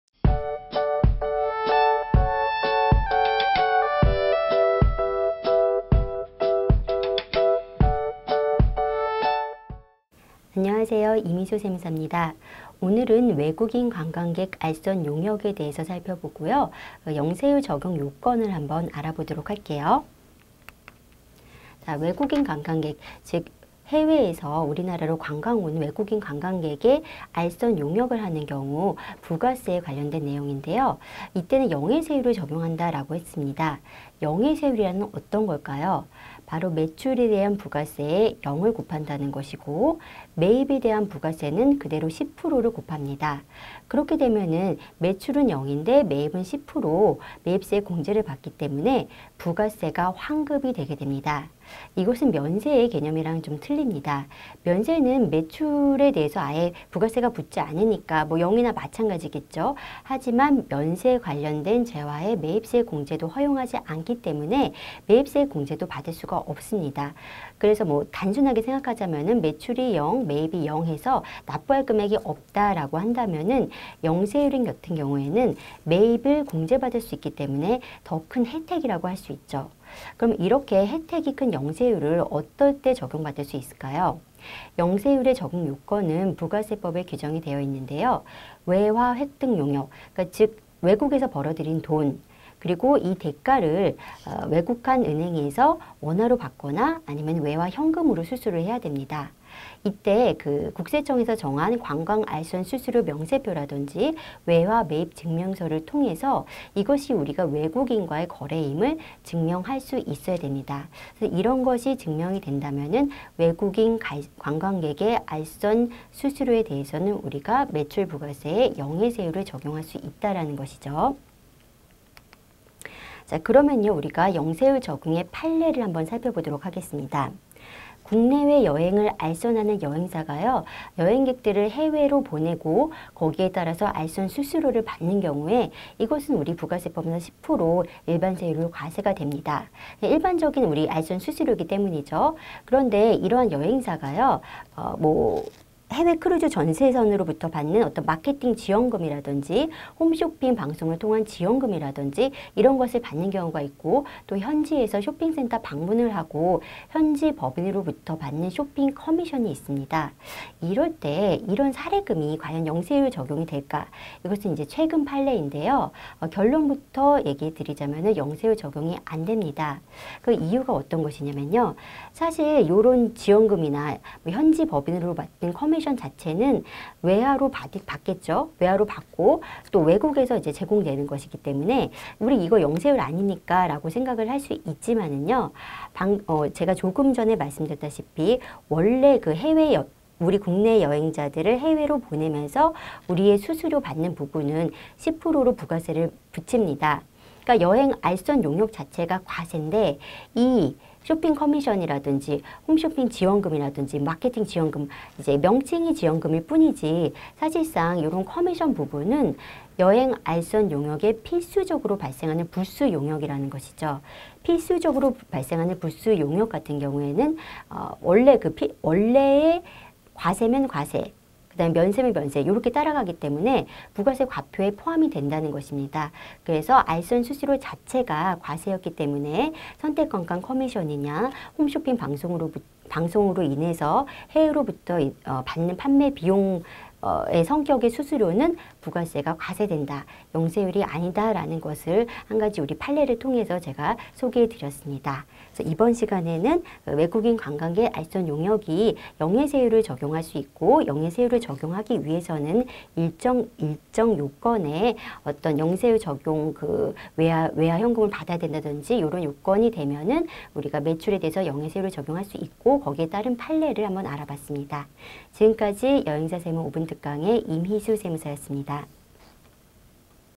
#9 외국인 관광객 알선용역 - 임희수 세무사의 5분특강
9.외국인 관광객 알선용역
- 외국관광객 알선수수료 영세율 적용요건은?